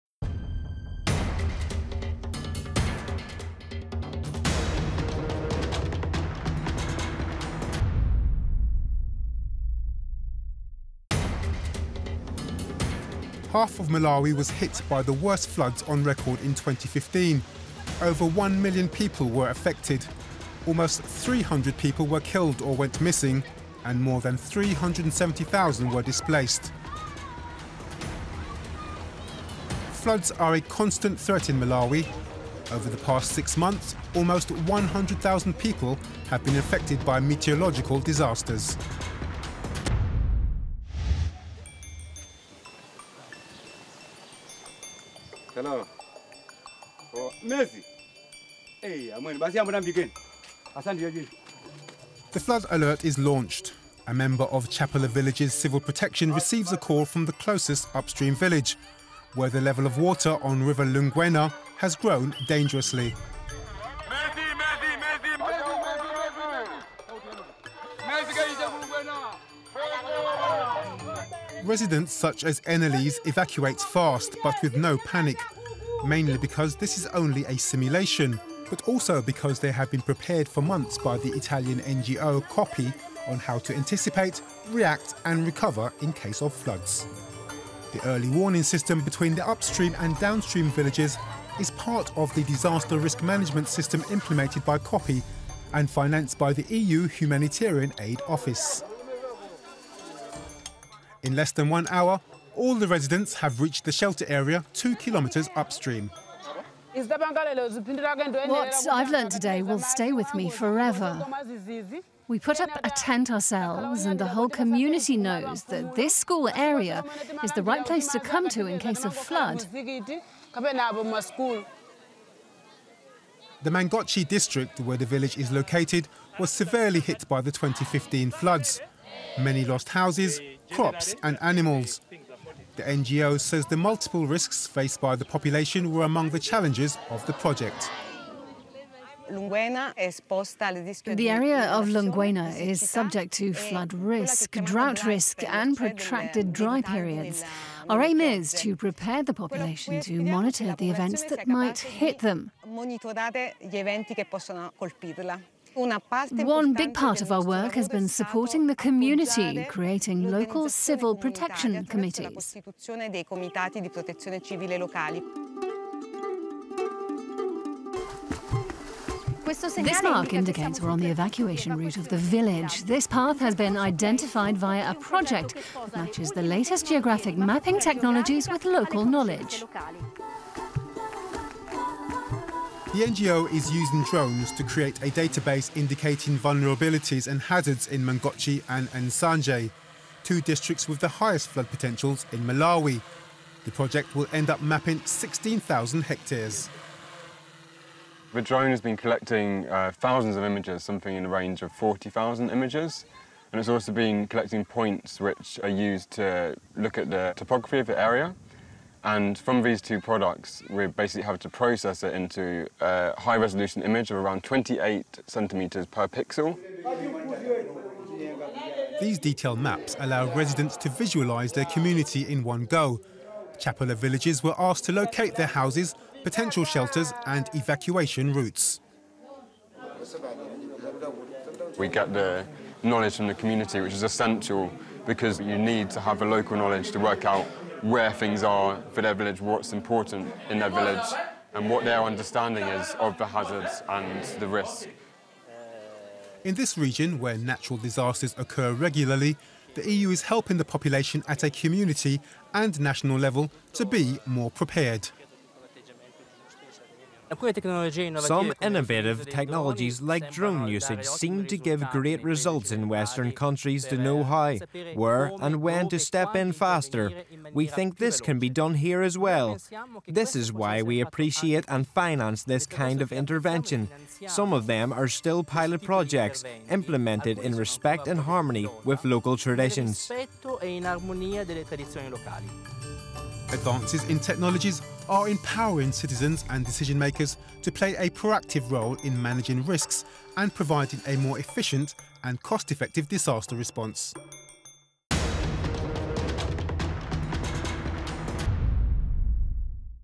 EU-funded drones help limit the impact of natural disasters in Malawi
Floods are a constant threat in Malawi. In 2015, half of the country was hit by the worst floods on record, affecting over one million people.

To help manage the risk of disasters and limit their impact, the humanitarian organisation COOPI implements early warning systems and regular evacuation exercises with the help of EU aid funding.

Thanks to EU support, COOPI is also using drones to create a database of vulnerabilities and hazards in the districts with the highest flood potential in Malawi. The drones collect thousands of images that are later transformed into detailed maps which allow residents to locate their houses, potential shelters, and evacuation routes; an exercise that benefits both scientists and locals.